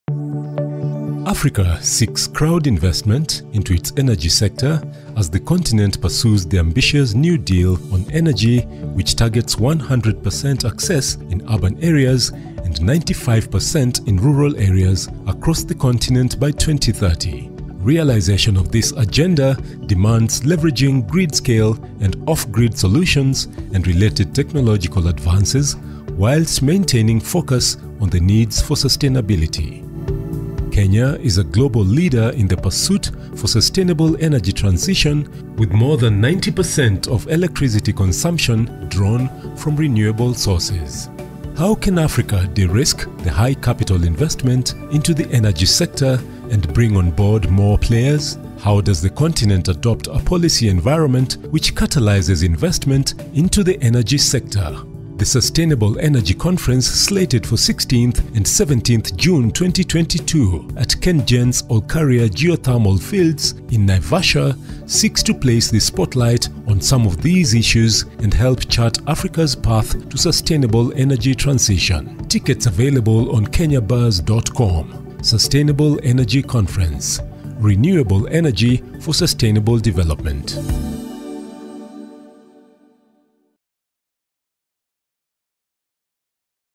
The Sustainable Energy Conference
Africa is a renewable energy continent,this potential is the cornerstone of enhancing energy access & industrialisation.
Join us at the Sustainable Energy Conference for insightful dialogues on reliable & sustainable modern energy for Africa.